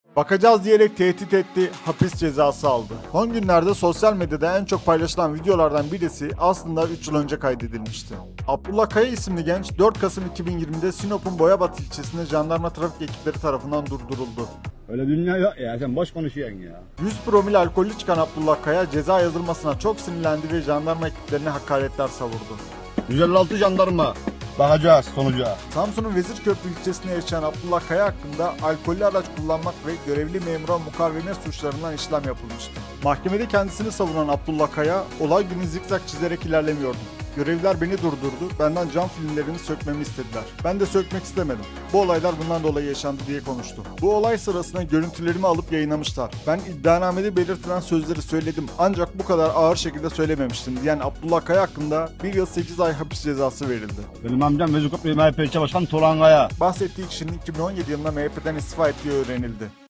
🚨'BAKACAZ' DEDİ HAPİS CEZASI ALDI! 🚔'Civata Reis' Abdullah Kaya'nın hikâyesi
👮‍♂️Son günlerde sosyal medyada en çok paylaşılan videolardan birisi aslında 3 yıl önce kaydedilmişti. Abdullah Kaya isimli genç, 4 Şubat 2020'de Sinop'un Boyabat ilçesinde jandarma trafik ekipleri tarafından durduruldu. 100 promil alkollü çıkan Abdullah Kaya, ceza yazılmasına çok sinirlendi ve jandarma ekiplerine hakaretler savurdu.

😑Samsun'un Vezirköprü ilçesinde yaşayan Abdullah Kaya hakkında 'Alkollü araç kullanmak' ve 'Görevli memura mukavemet' suçlarından işlem yapılmıştı. Mahkemede kendisini savunan Abdullah Kaya, "Olay günü ben zigzag çizerek ilerlemiyordum. Görevliler beni durdurdu. Benden cam filmlerini sökmemi istediler. Bende sökmek istemedim. Bu olaylar bundan dolayı yaşandı" diye konuştu.

📹Kaya, "Bu olay sırasında görüntülerimi alıp yayınlamışlar. Ben iddianamede belirtilen sözleri söyledim ancak bu kadar ağır bir şekilde söylememiştim." ifadelerini kullandı. Yapılan yargılama sonrası mahkeme Abdullah Kaya hakkında 'Görevi Yaptırmamak İçin Direnme' ve 'Hakaret' suçlarını işlediği gerekçesiyle toplamda 1 yıl 8 ay hapse mahkûm edilmesine karar verildi.

Abdullah Kaya, 2020'de kaydedilen görüntülerde, amcasının MHP ilçe başkanı olduğunu iddia etse de bahsettiği kişinin 2017 yılında MHP'den istifa ettiği öğrenildi.